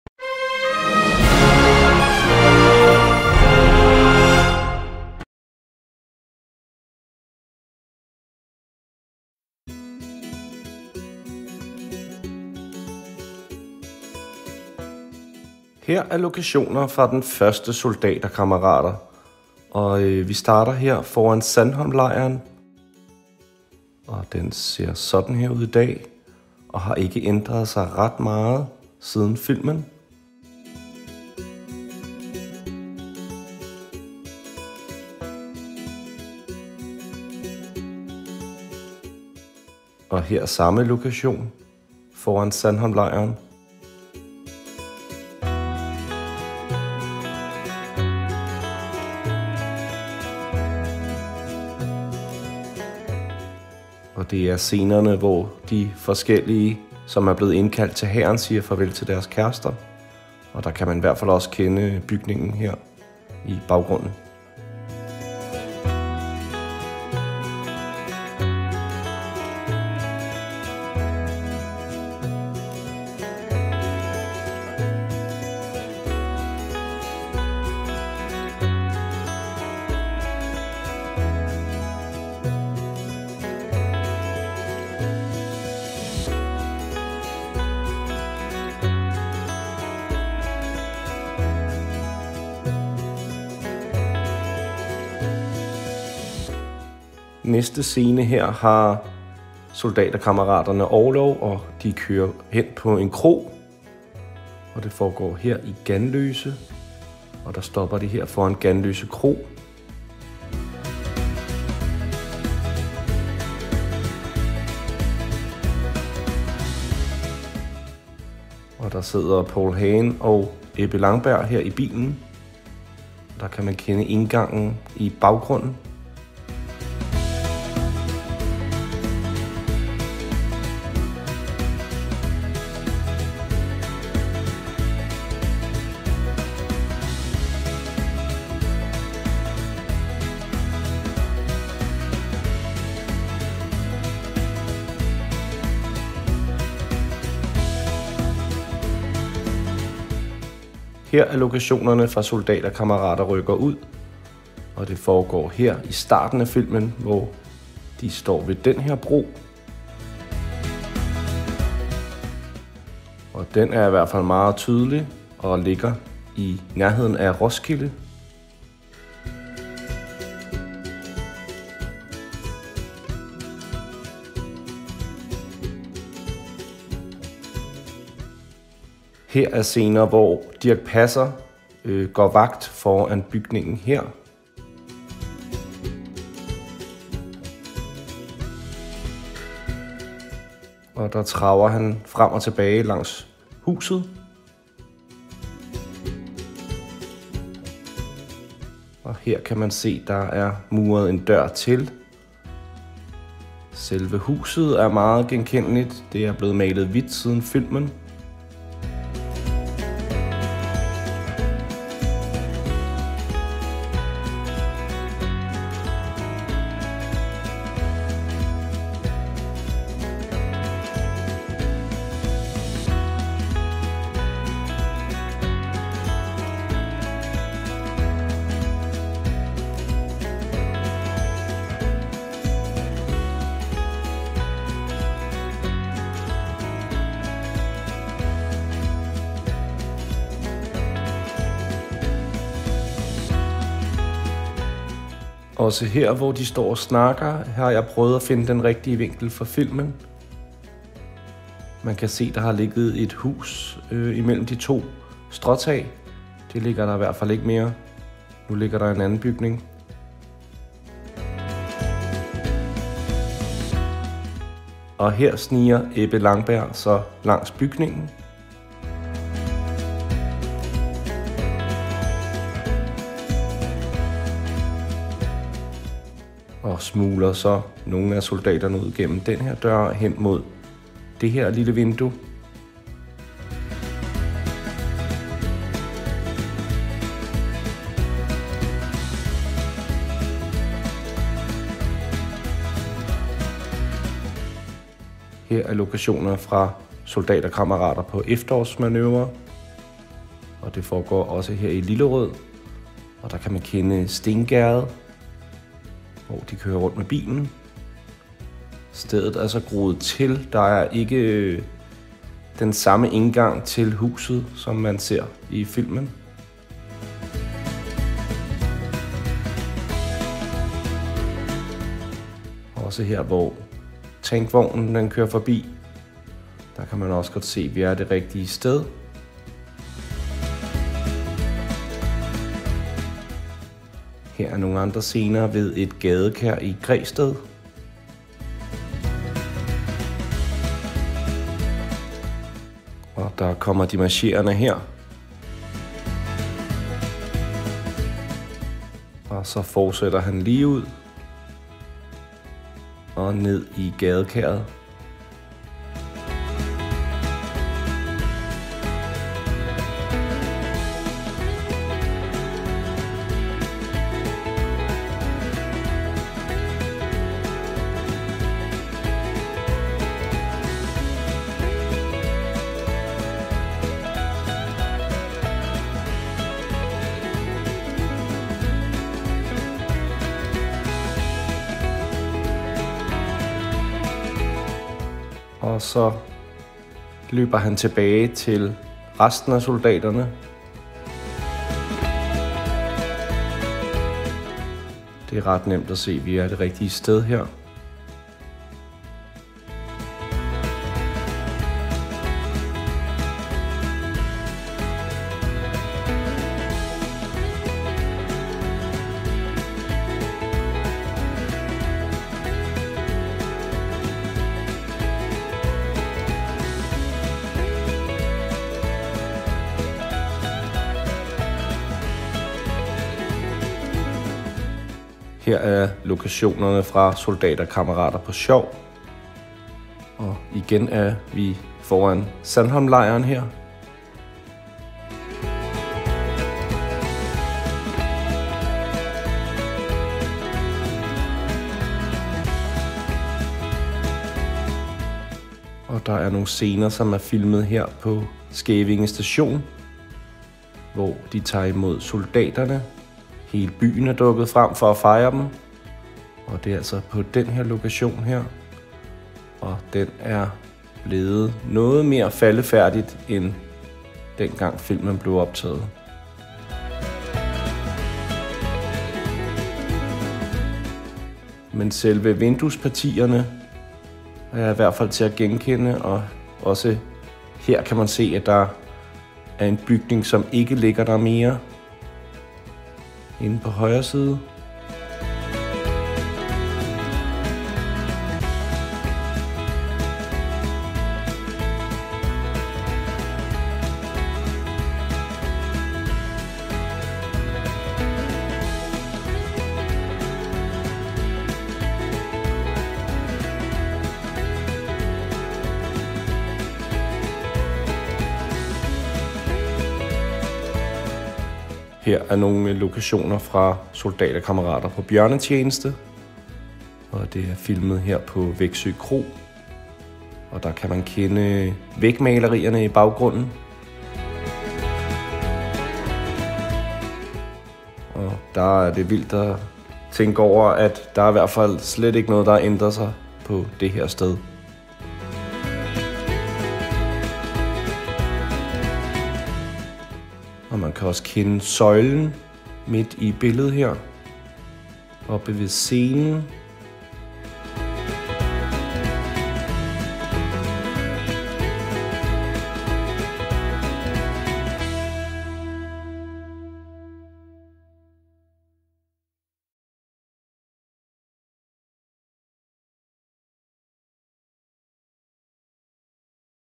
Filmlokationer - Soldater Kammerater Filmene - Den danske hær , gamle danske film .
Lokationer fra Soldaterkammerater, med Klaus Pagh , Preben Kaas , Poul Hagen , Dirch Passer og Carl Ottosen..

Vi Kommer hele Nordsjælland rundt i denne Video , håber i kan Lide Den.